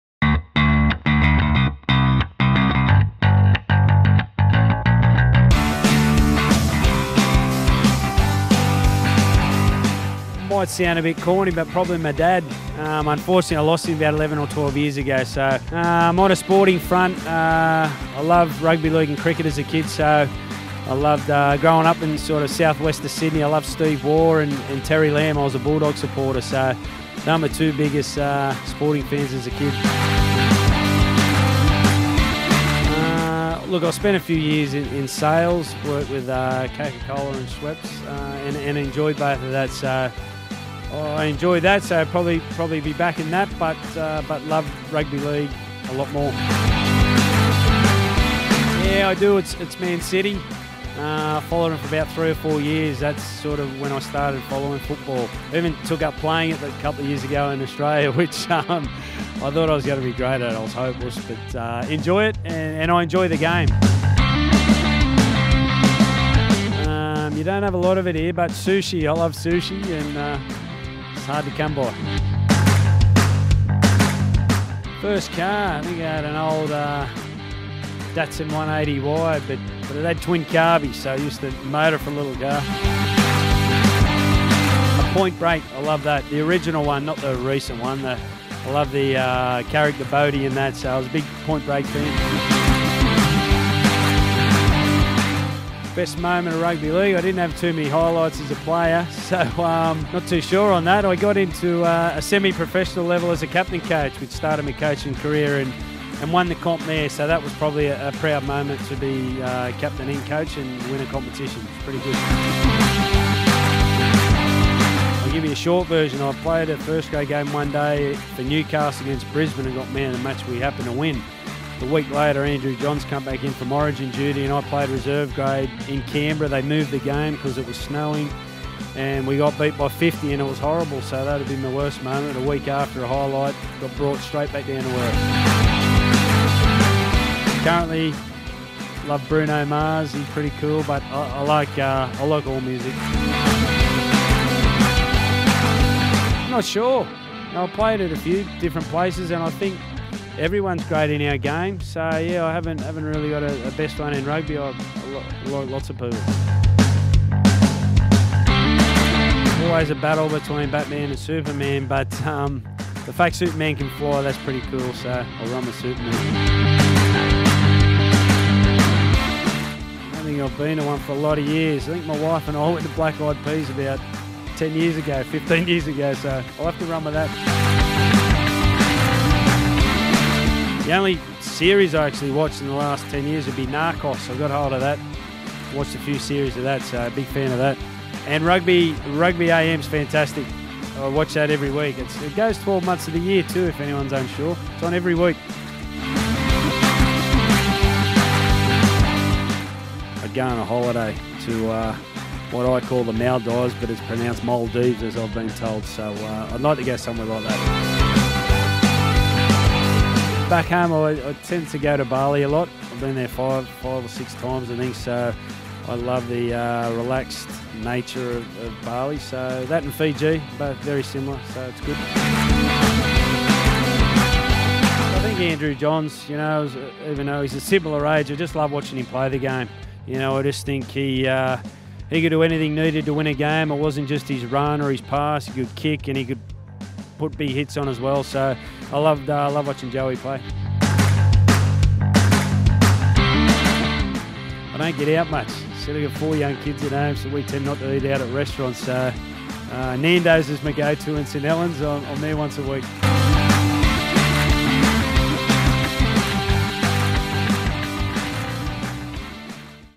Getting To Know You: Justin Holbrook
Following their superb start to the season, we learn a little bit more about St Helens head coach Justin Holbrook.